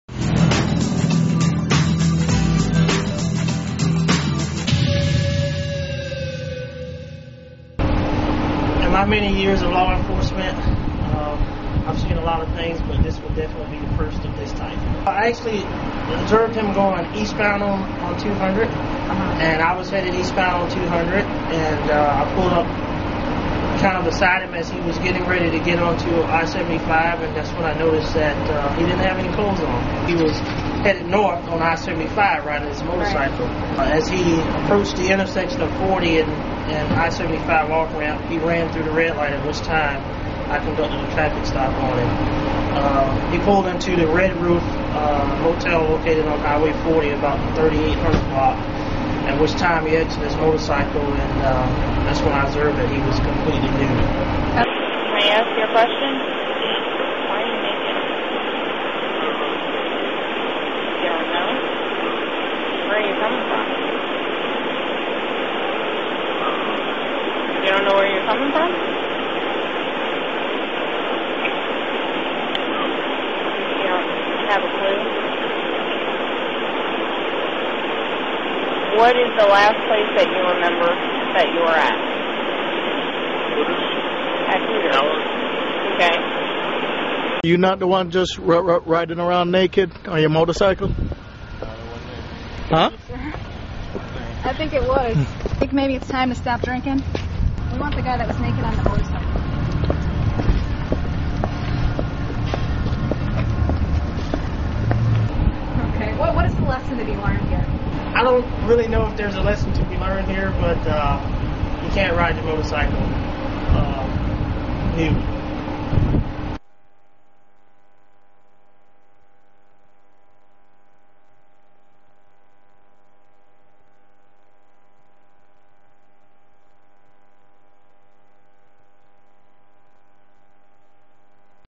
Naked Biker Arrested
J Dante Krauss was arrest early Tuesday morning when Marion County Sheriff's deputy Mike Rolls spotted him riding his motorcycle naked.